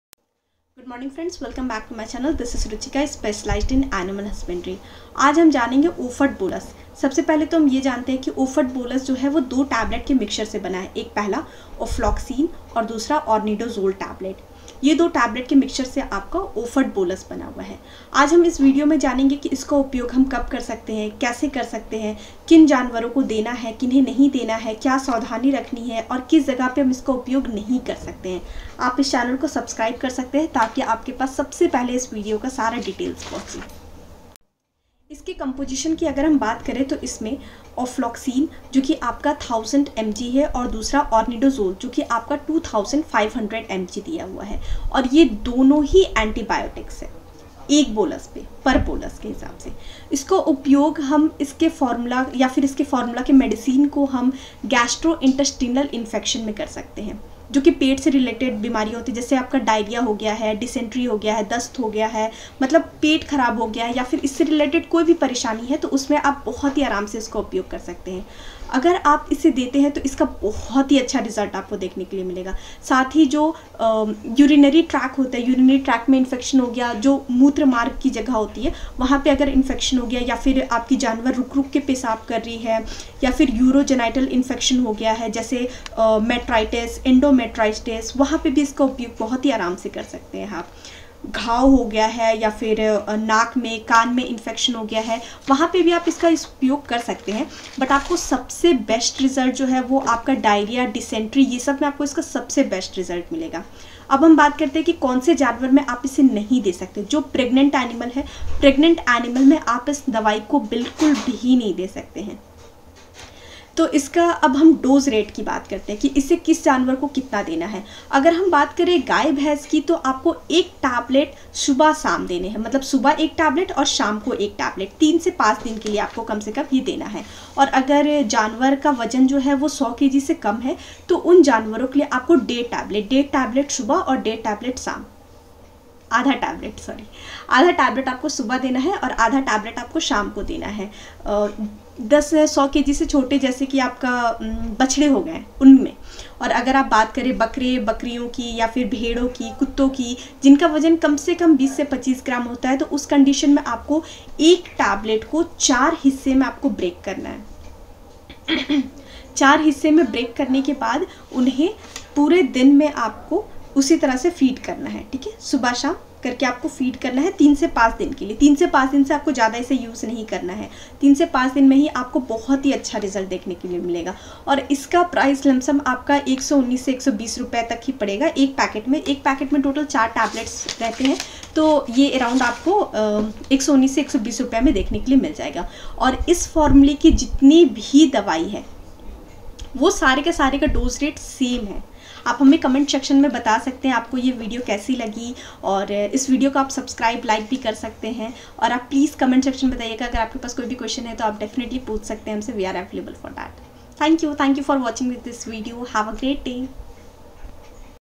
Oford Bolus ! ofloxacin and ornidazole tablet| डायरिया दस्त की दवा Veterinary Medicine
गाय, भैस -  1 टेबलेट सुबह -शाम
बकरी , भेड़, कुत्तों  - 1/2 टेबलेट सुबह -शाम